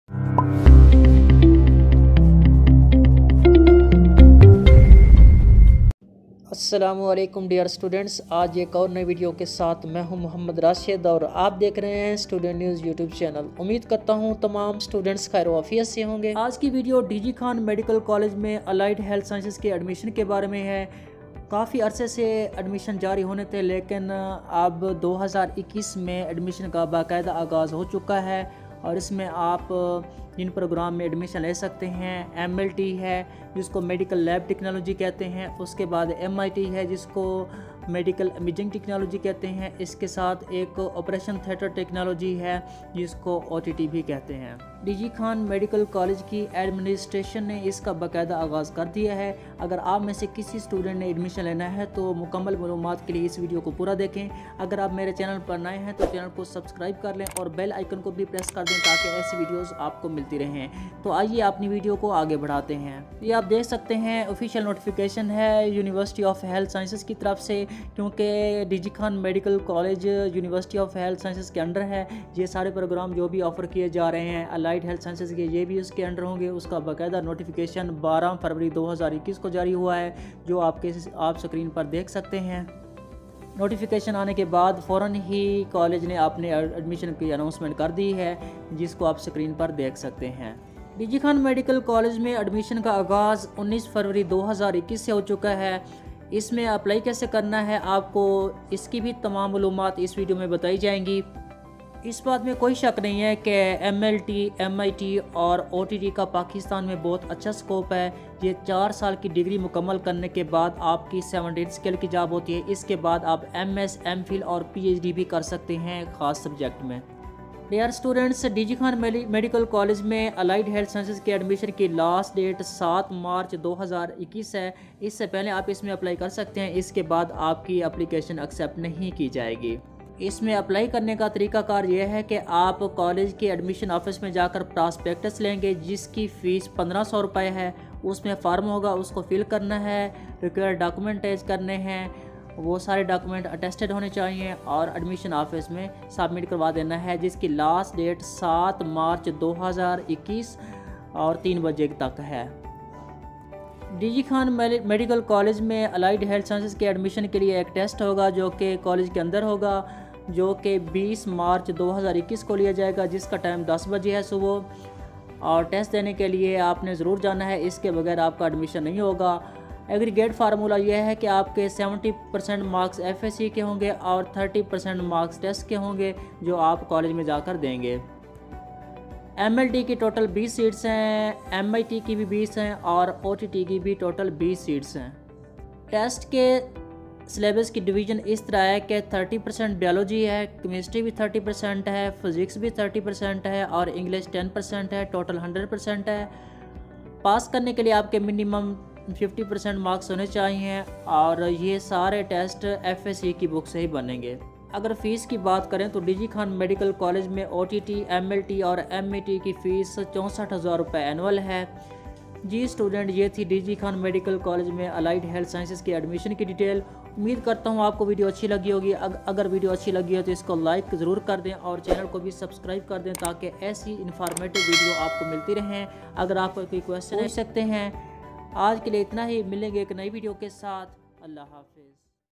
Dera Ghazi Khan Medical College MLT,OTT & MIT admission 2021||New MLT admission 2021|Students News
Dera Ghazi Khan is a mid-country city located on the junction of all the four provinces of Pakistan i.e. Sindh, Punjab, Balochistan, and Khayber Pukhtunkhwa. Due to its location, it has been once in the list of prospective capitals of Pakistan. It is has beautiful and unique landscape including River Indus, green fields, Pachad Desert and the Sulaiman Mountains.D.G. Khan Medical College was established in 2010.Initially the classes were started at Quaid-e-Azam Medical College, Bahawalpur, which were shifted to the D.G.Khan on November 28, 2011. The Government of the Punjab has allocated 522 kanals of land for the construction of regular academic block and hostel building and expansion of attached 346-bed DHQ Hospital ,where all the major medical and surgical specialties including, Medicine, Surgery, Pediatrics, Gynecology, Eye, ENT, etc., are available.